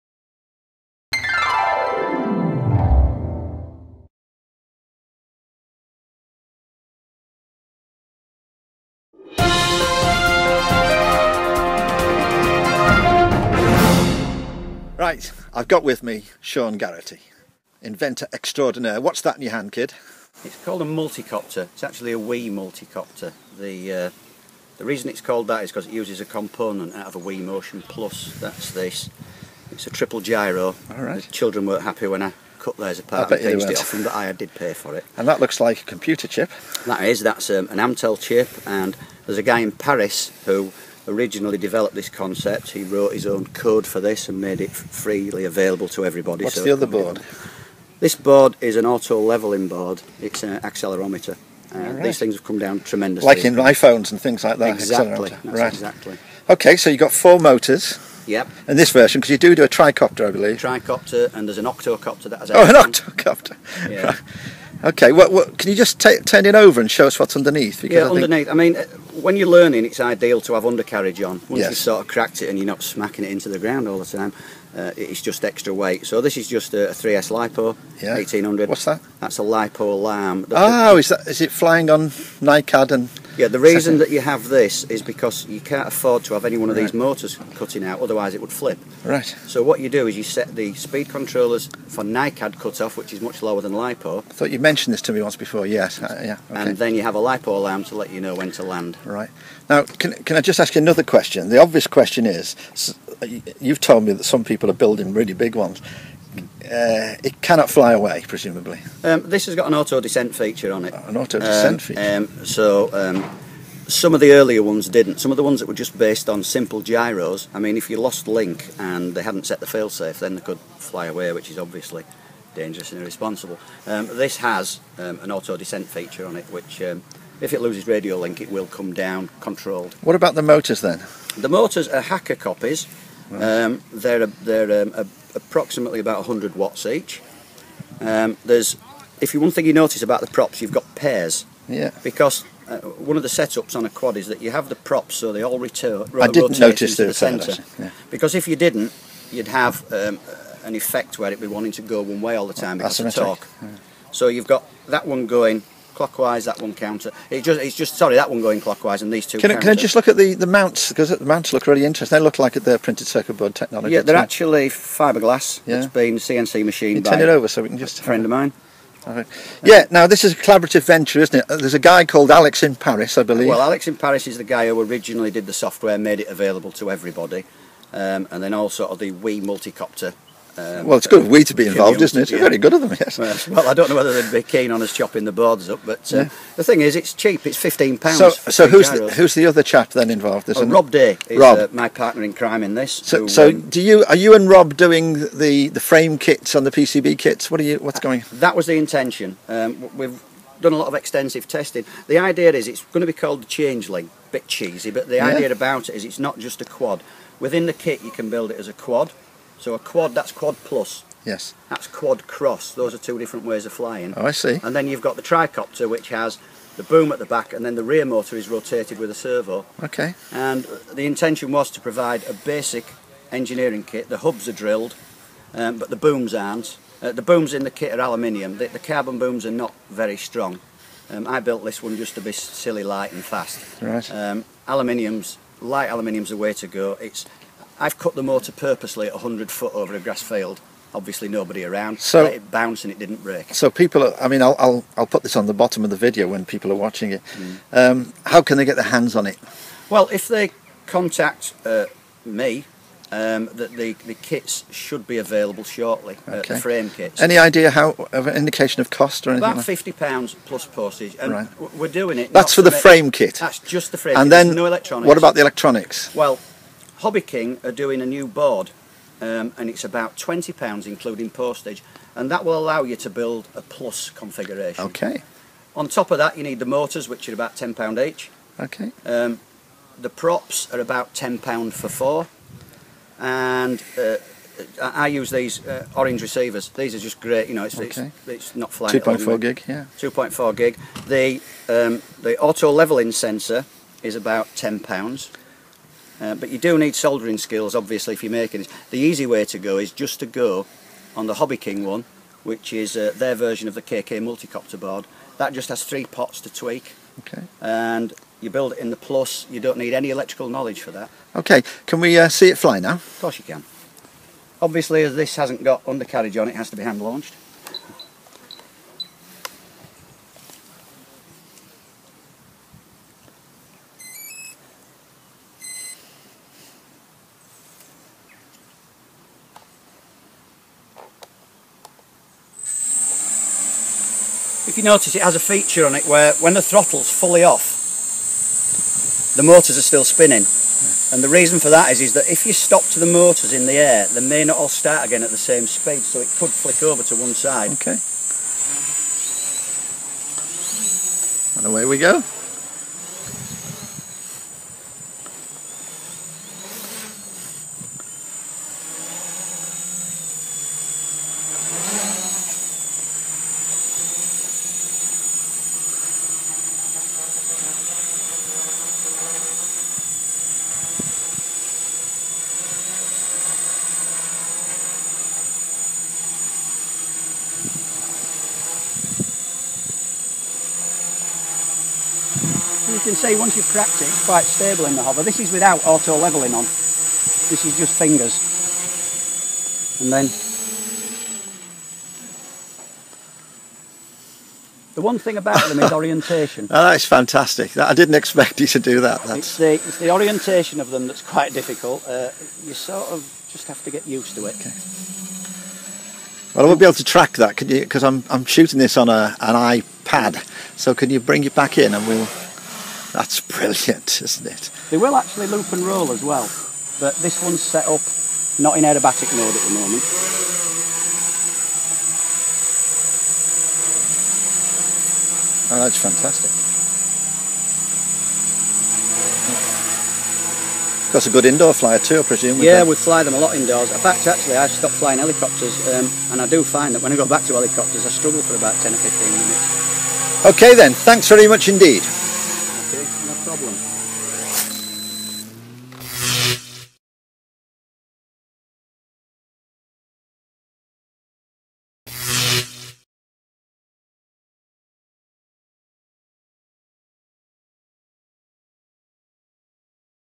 Changeling Multicopter
Shaun Garrity and his Changeling build-it-yourself Multicopter